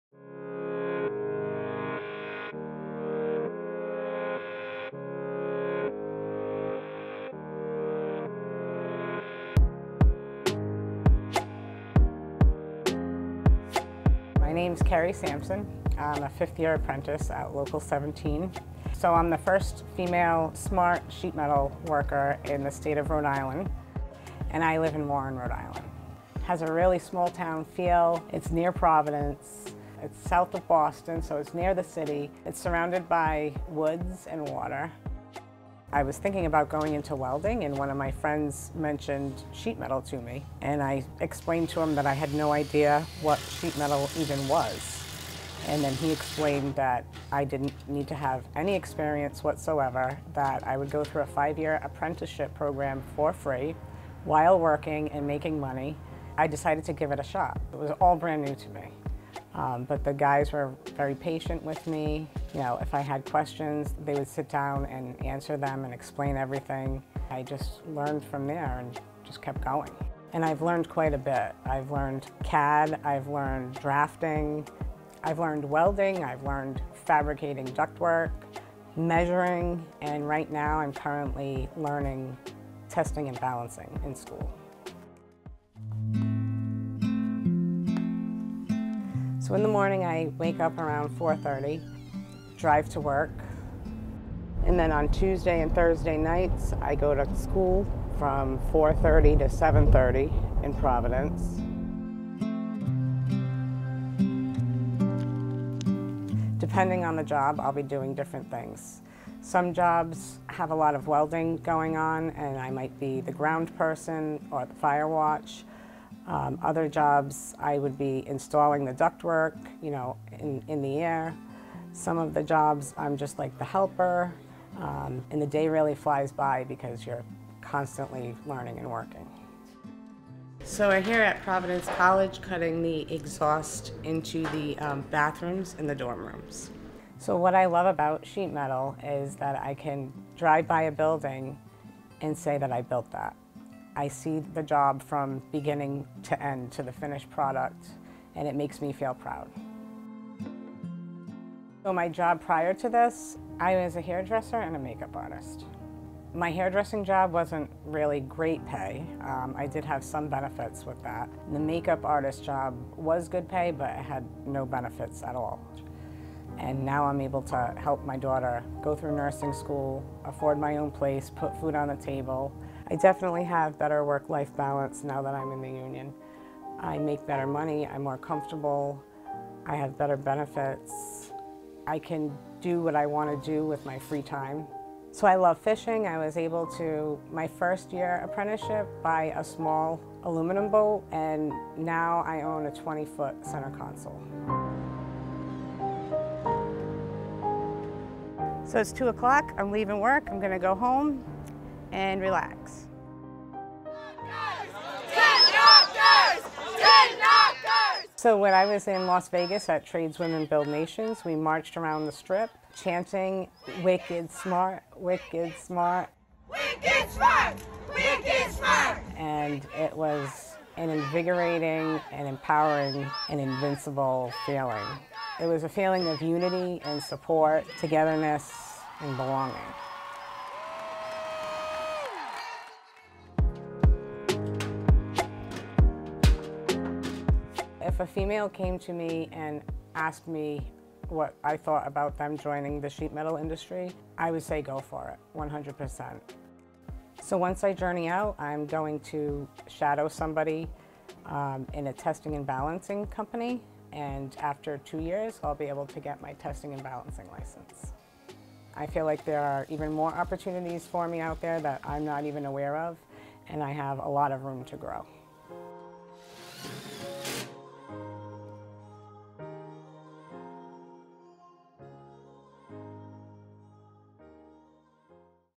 Member Profile: Sheet Metal Apprentice Kerry Sampson
Local 17 5th-year apprentice Kerry Sampson is the first female SMART sheet metal member in the state of Rhode Island. Watch as she discusses how she got into the trade, transitioning from juggling non-union hairdresser and make-up artist jobs with minimal to no benefits, to a union job with great pay and benefits, a real career path, better work/life balance and retirement security.